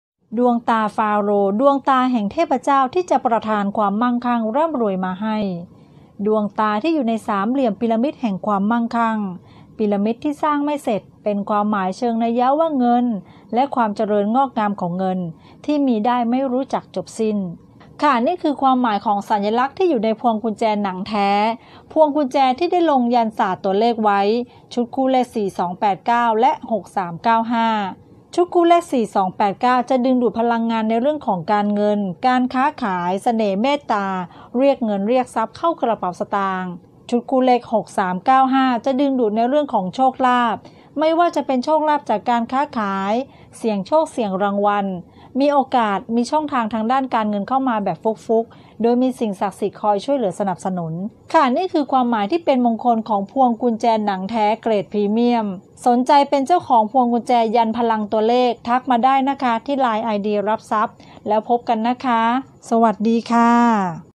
ดวงตาฟาโรห์ สามเหลี่ยมพีระมิด 4289 6395 พวงกุญแจของชำร่วย ของผู้ใหญ่
พวงกุญแจหนังแท้ ลงยันต์ศาสตร์ตัวเลข ชุด 4289 และ 6395 โลโก้ดวงตาฟาโรห์ และพีระมิดแห่งความมั่งคั่ง พวงกุญแจหนังแท้ที่เป็นมงคลเหมาะสำหรับให้ผู้ใหญ่ ในงานแต่งงาน หรืองานมงคลต่างๆ